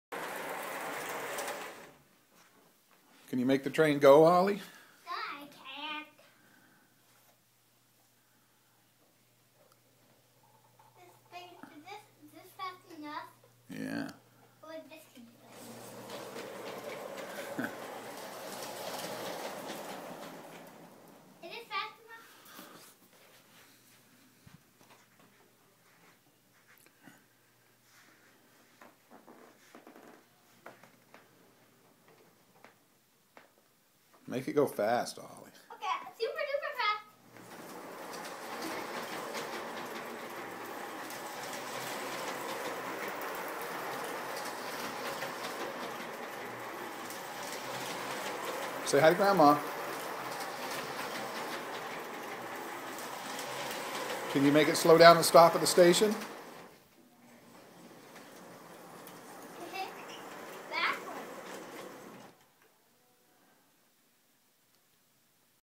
Oliver the Train Engineer
Oliver running his 40 year old L.G.B. train